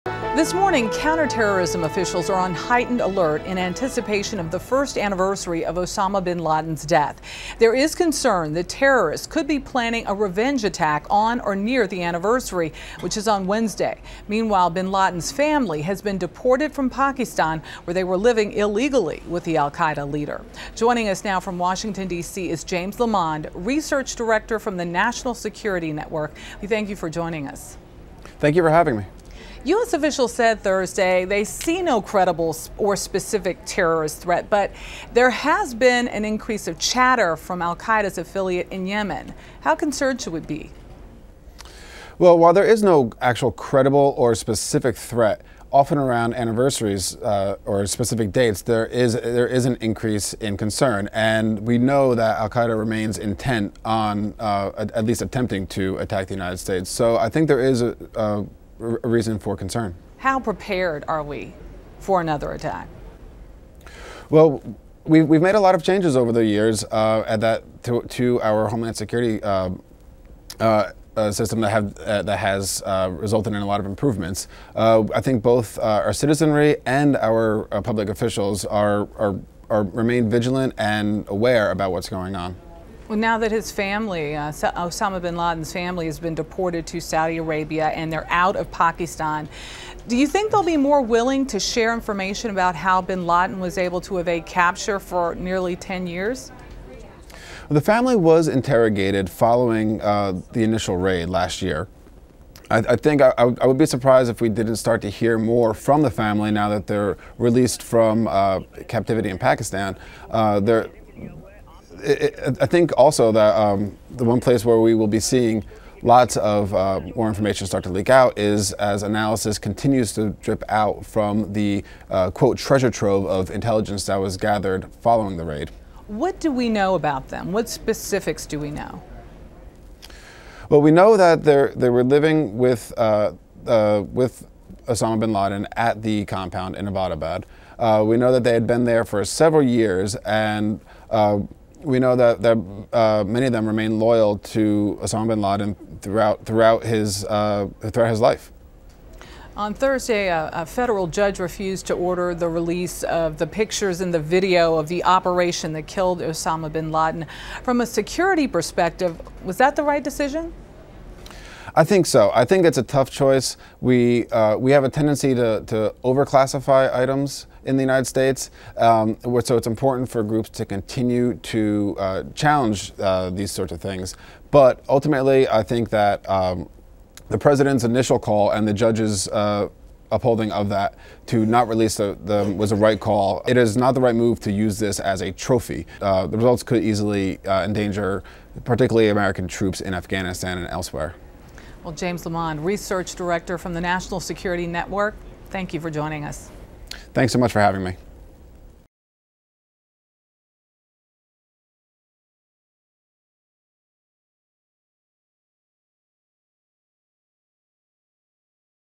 Anniversary Attacks
James Lamond, Research Director at the National Security Network discusses the latest terror alert involving the anniversary of the death of Osama Bin Laden.